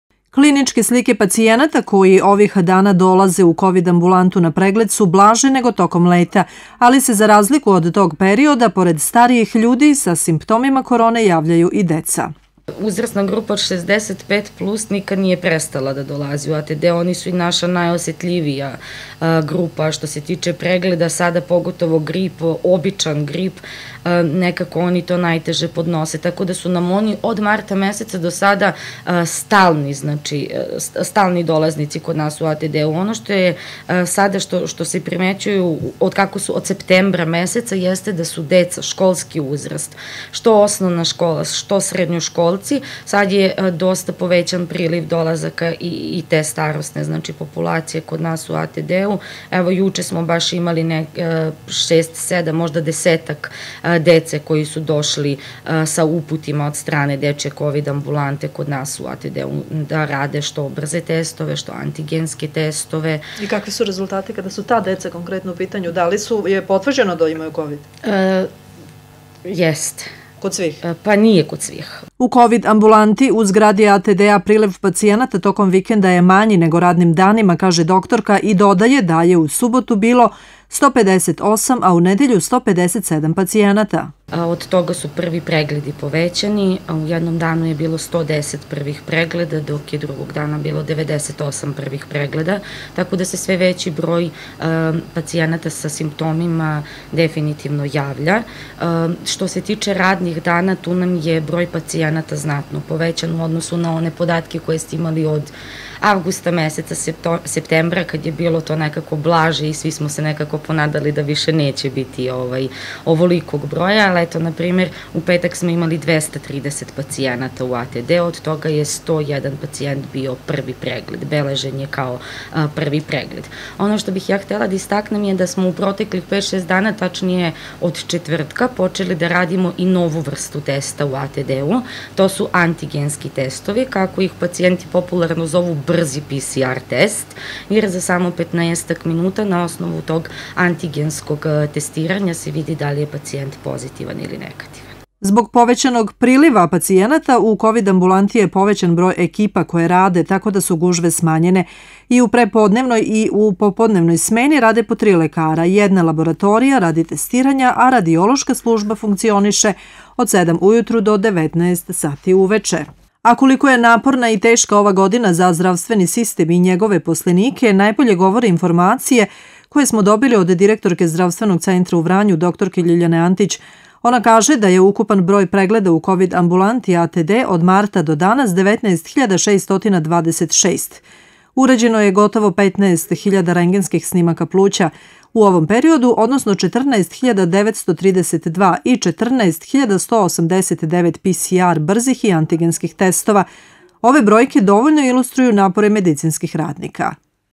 Klinicka slika osoba koje se javljaju u kovid ambulanti / RTV Vranje / 23 11 2020
Javlja se veci broj mladih / RTV Vranje / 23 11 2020